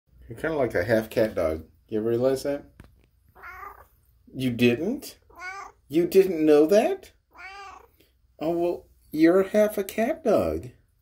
Kitty Conversations: Half a Cat-Dog
A small clip of a conversation with my cat.